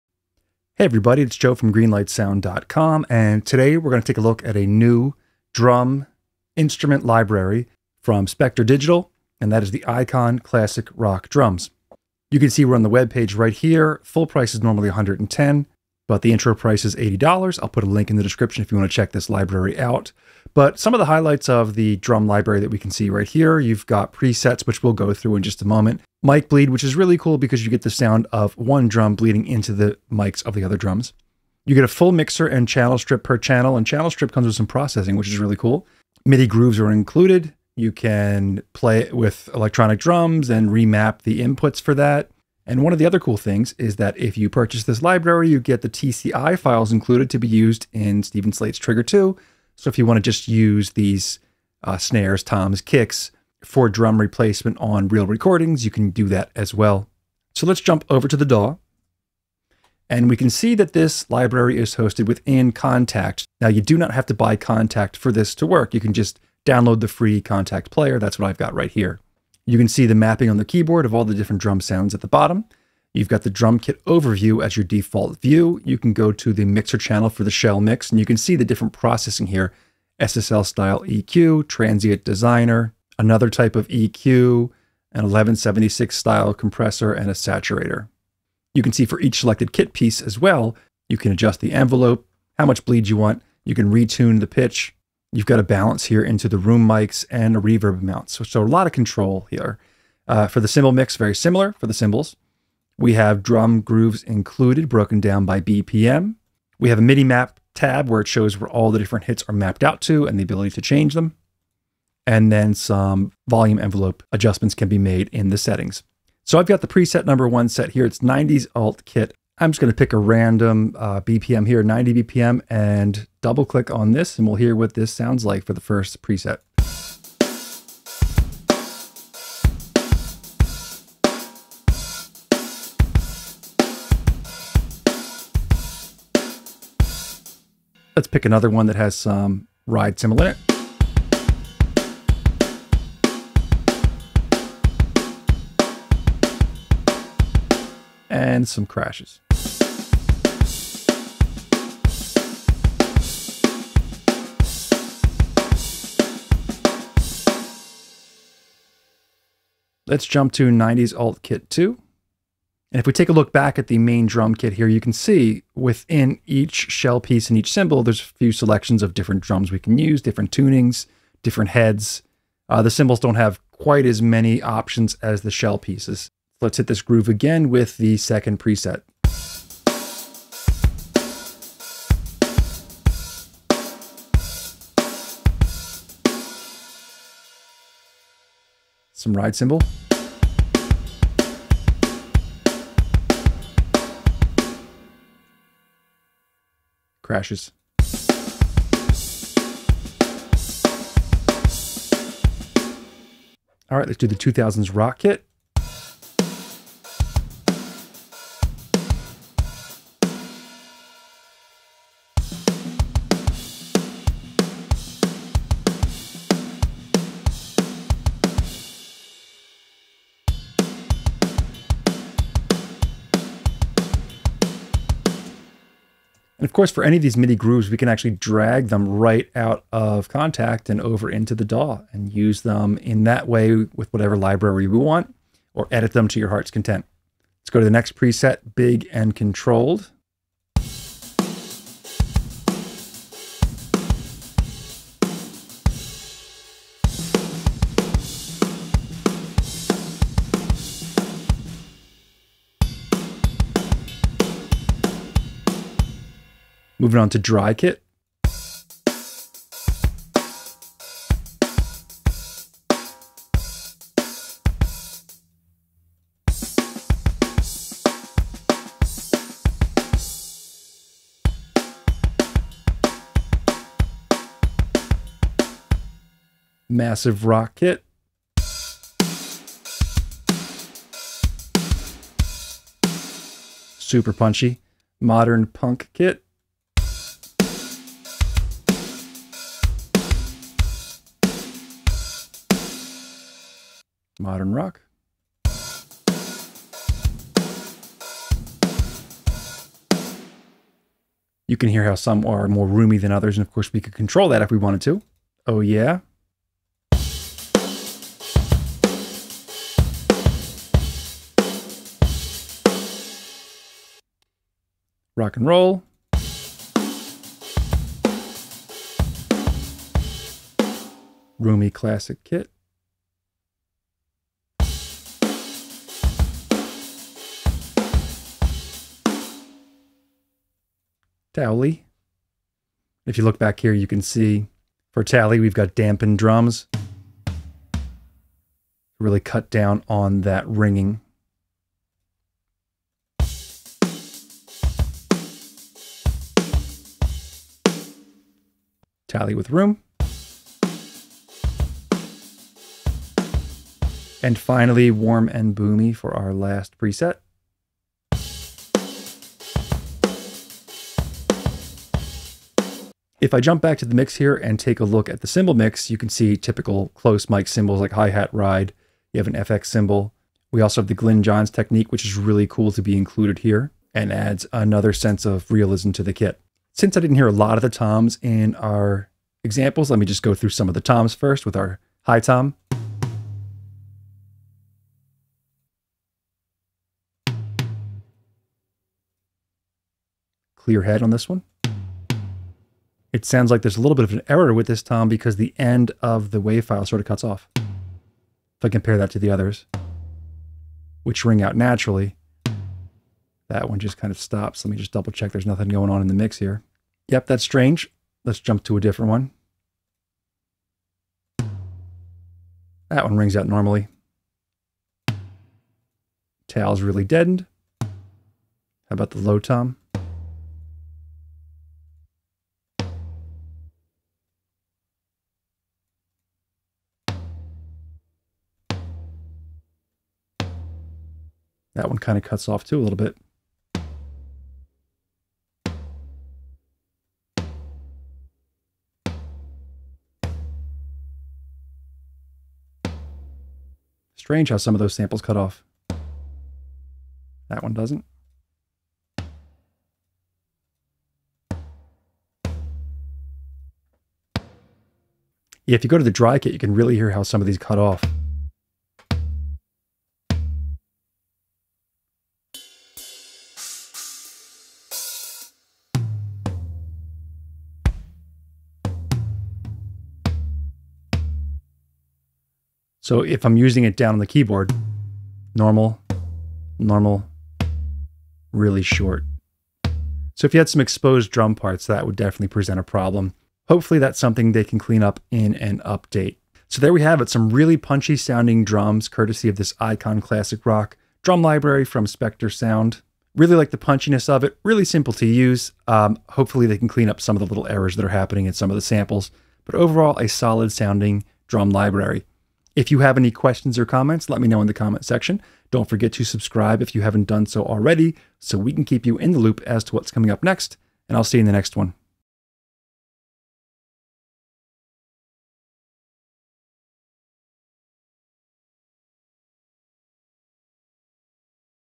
ICON Drums Classic Rock | New Drum Library from Spectre Digital
0:00 - Intro
1:09 - The Interface
1:59 - The Sounds
6:16 - The Toms & Issues
8:06 - Conclusion